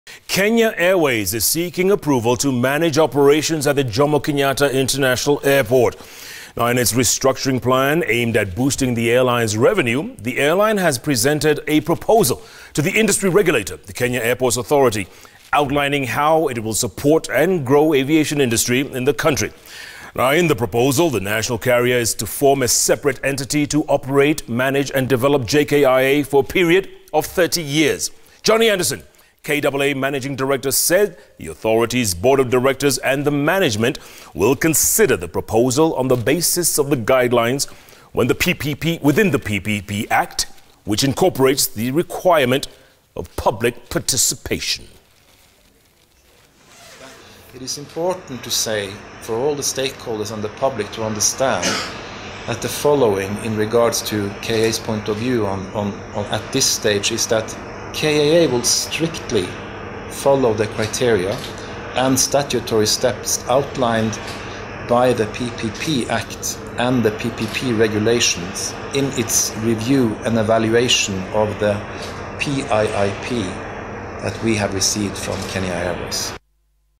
Kenya Airways seeks for approval to manage JKIA
Kenya Airways is seeking approval to manage operations at the Kenyatta International Airport. In its restructuring plan aimed at boosting the airlines’ revenue, the airline has presented a proposal to the industry regulator Kenya airports authority outlining how it will support and grow aviation industry.